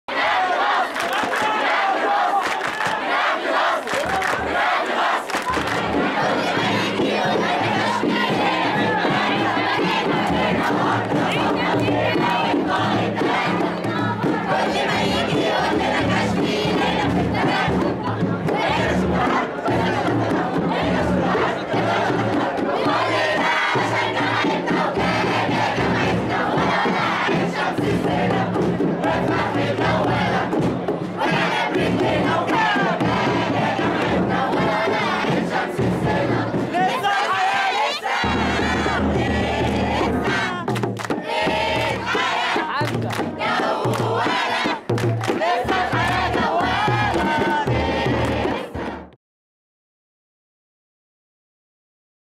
طبل ورقص في احتفال طلاب جامعة عين شمس بأول أيام الدارسة
تصوير: إبتهال أسامة